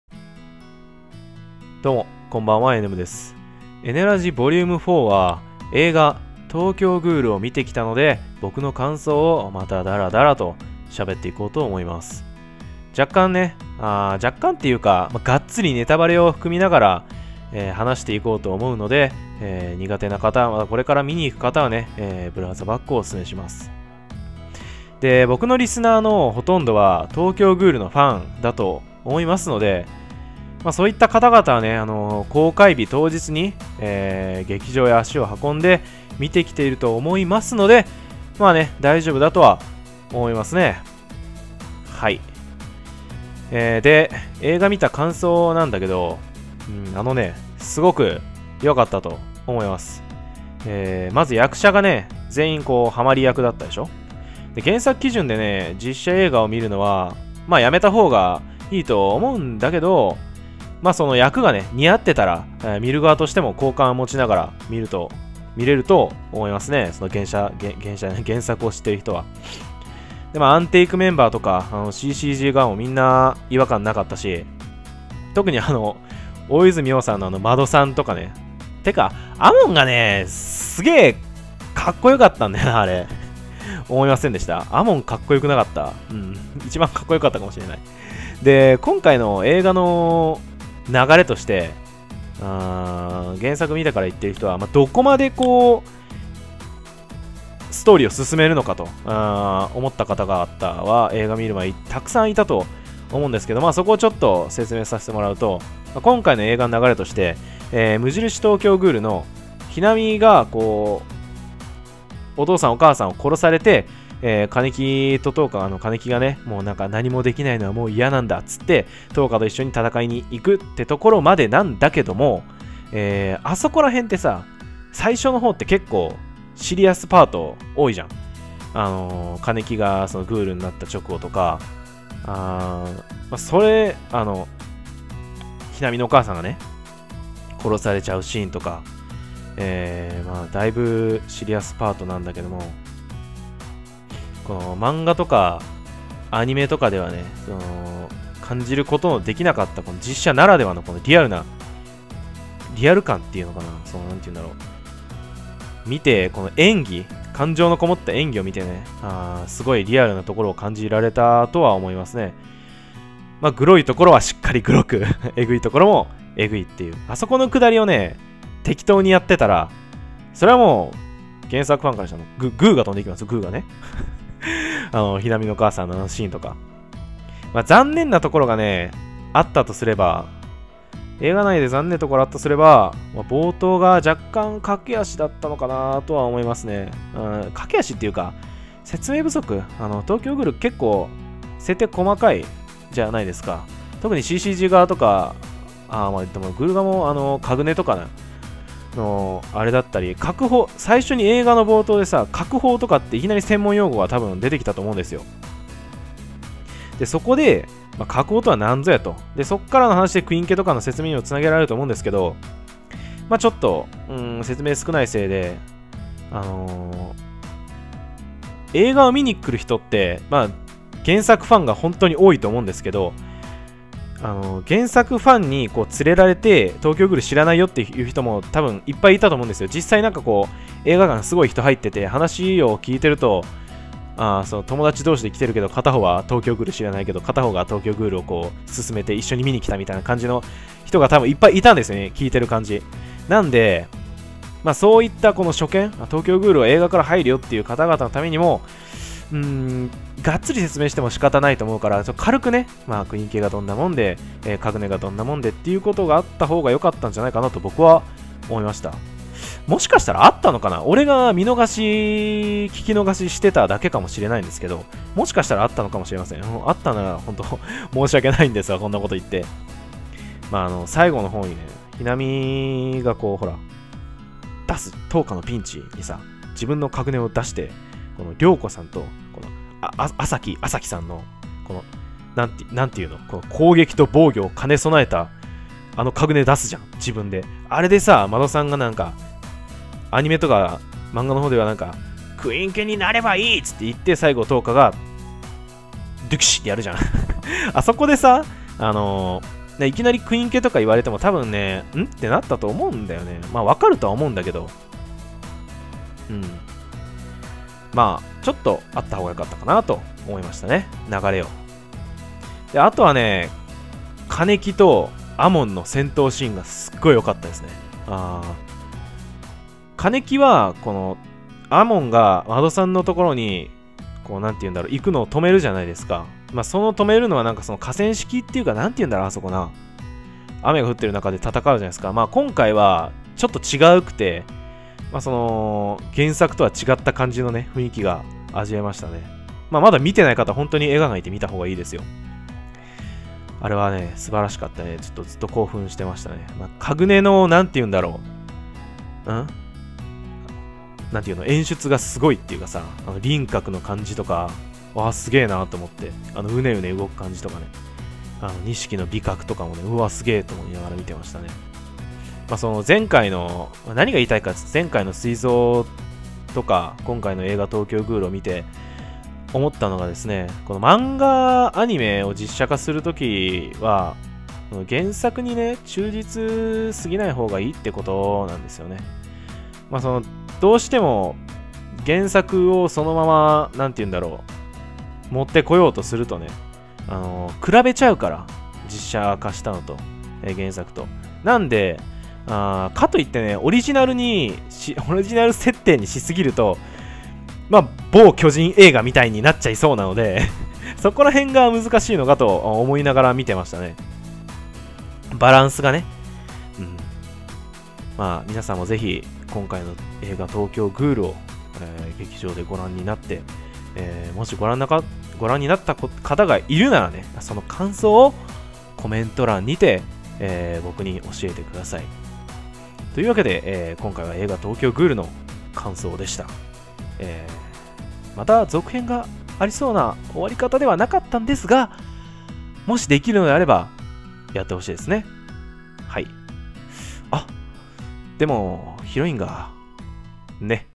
【えねラジ】Vol.4 映画「東京喰種 トーキョーグール」そこにはリアルがあった。
これからも東京喰種が皆さんに愛される作品でありますように